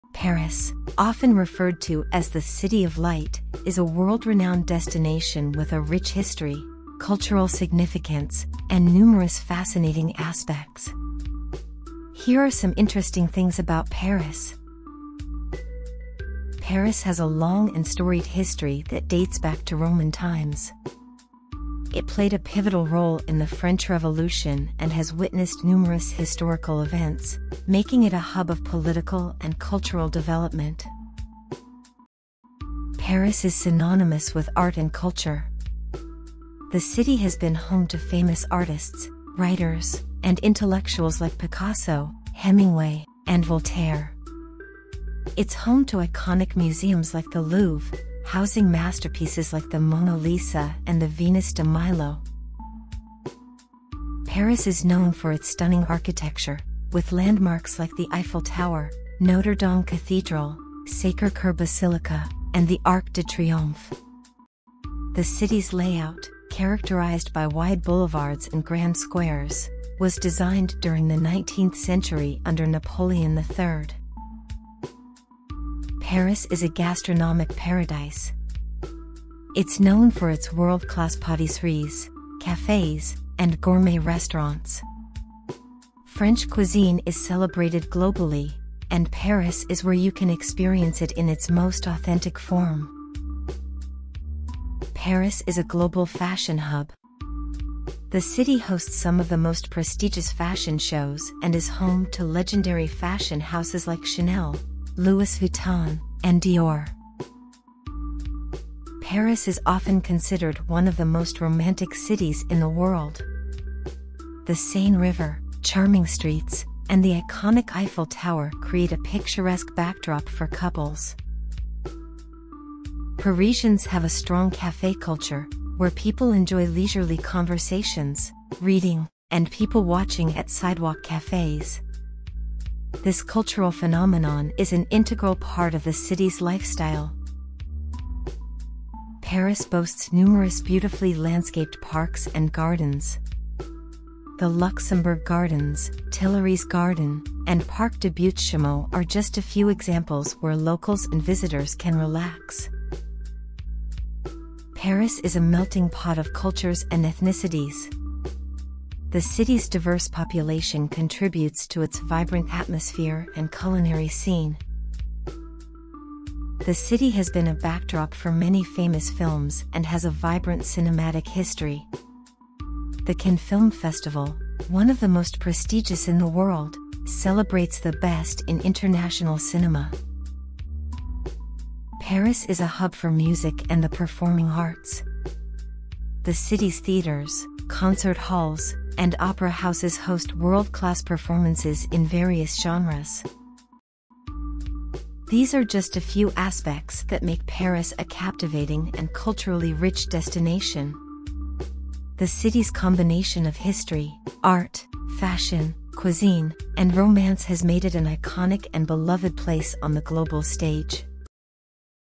Awesome tourist destination to City of Light, Paris.
Paris, often referred to as the "City of Light," is a world-renowned destination with a rich history, cultural significance, and numerous fascinating aspects. Here are some interesting things about Paris.

Paris has a long and storied history that dates back to Roman times. It played a pivotal role in the French Revolution and has witnessed numerous historical events, making it a hub of political and cultural development.

Paris is synonymous with art and culture. The city has been home to famous artists, writers, and intellectuals like Picasso, Hemingway, and Voltaire. It's home to iconic museums like the Louvre, housing masterpieces like the Mona Lisa and the Venus de Milo.

Paris is known for its stunning architecture, with landmarks like the Eiffel Tower, Notre-Dame Cathedral, Sacré-Cœur Basilica, and the Arc de Triomphe. The city's layout, characterized by wide boulevards and grand squares, was designed during the 19th century under Napoleon III.

Paris is a gastronomic paradise. It's known for its world-class patisseries, cafes, and gourmet restaurants. French cuisine is celebrated globally, and Paris is where you can experience it in its most authentic form.

Paris is a global fashion hub. The city hosts some of the most prestigious fashion shows and is home to legendary fashion houses like Chanel, Louis Vuitton, and Dior.

Paris is often considered one of the most romantic cities in the world. The Seine River, charming streets, and the iconic Eiffel Tower create a picturesque backdrop for couples.

The city has inspired countless writers, and many famous literary works are set in Paris. Shakespeare and Company, a renowned English-language bookstore, has been a gathering place for writers and readers for decades.

Parisians have a strong café culture, where people enjoy leisurely conversations, reading, and people-watching at sidewalk cafes. This cultural phenomenon is an integral part of the city's lifestyle.

Paris boasts numerous beautifully landscaped parks and gardens. The Luxembourg Gardens, Tuileries Garden, and Parc des Buttes-Chaumont are just a few examples where locals and visitors can relax.

Paris is a melting pot of cultures and ethnicities. The city's diverse population contributes to its vibrant atmosphere and culinary scene.

The city has been a backdrop for many famous films and has a vibrant cinematic history. The Cannes Film Festival, one of the most prestigious in the world, celebrates the best in international cinema.

Paris is a hub for music and the performing arts. The city's theaters, concert halls, and opera houses host world-class performances in various genres.

These are just a few aspects that make Paris a captivating and culturally rich destination. The city's combination of history, art, fashion, cuisine, and romance has made it an iconic and beloved place on the global stage.